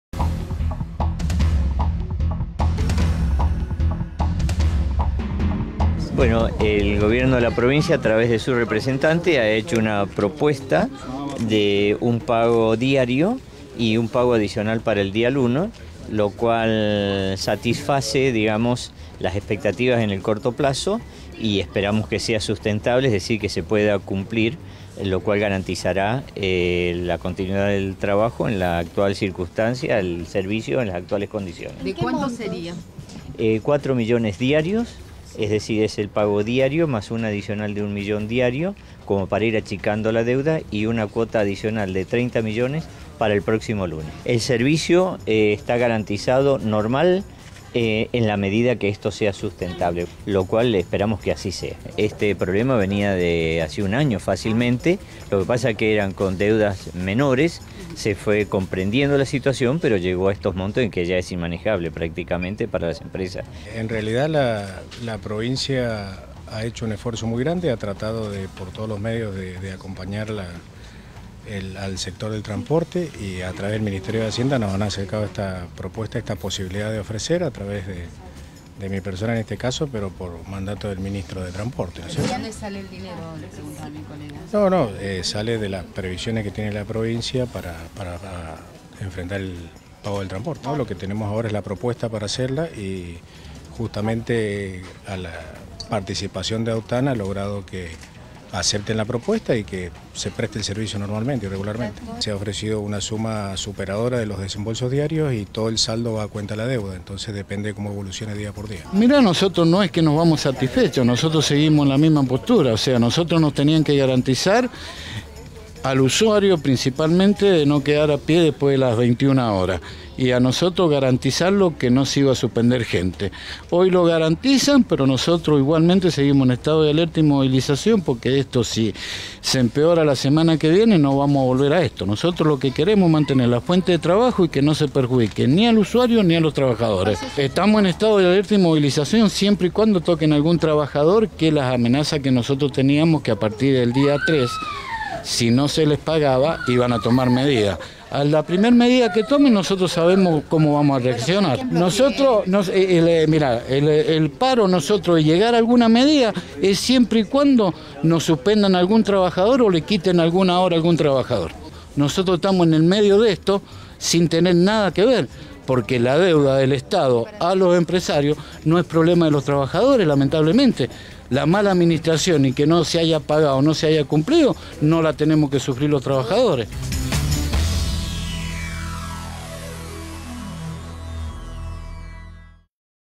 Deuda de transporte público: AUTAM arregló con el estado
El gobierno provincial presentó un plan de pago y AUTAM decidió frenar la suspensión del servicio